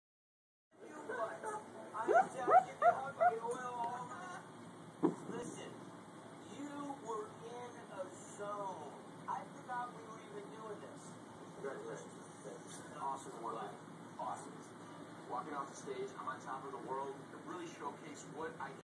Vizsla puppy sleep barking
This is Sasha, my female vizsla, the murmered squeeking you hear in the beginning is her,  she normally does this for long periods of time but I think she knew she was being filmed, she's a noisy slee